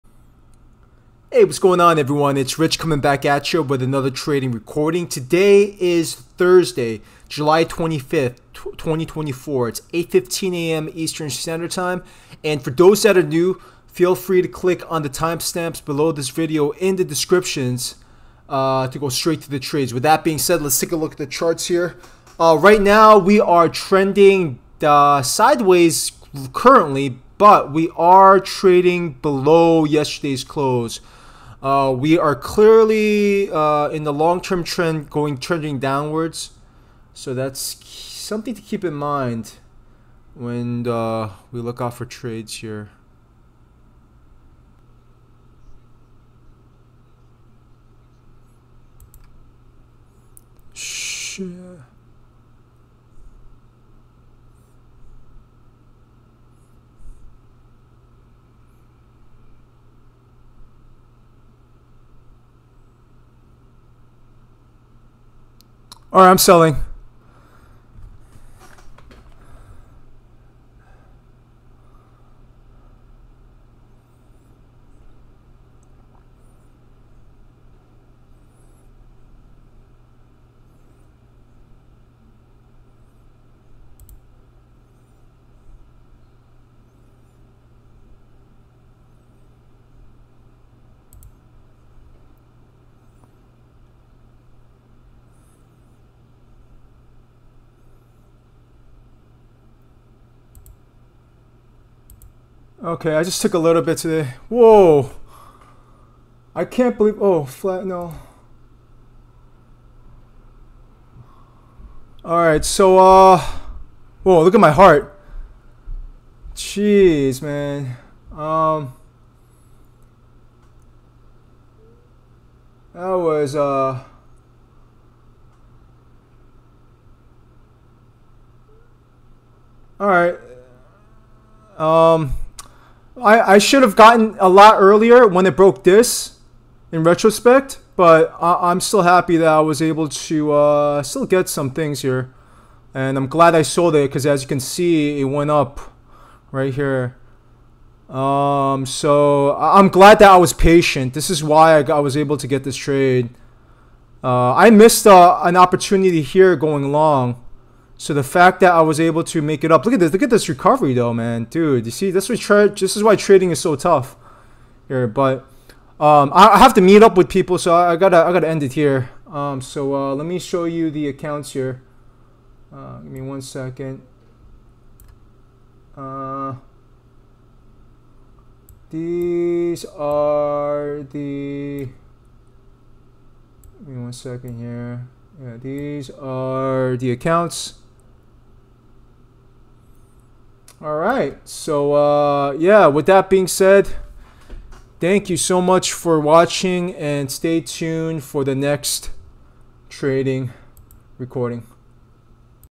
+$1,041.10 Profit 1 trade 7/25/24
1:06 First and only trade (win)
3:06 Showing my evaluation accounts.

1 trade, 1 win.

Missed a couple of trades today again, however I was able to be patient and find another trade.  I went short after a key level was broken in the short term and was able to grab a profit.  So far so good this week.

Recorded session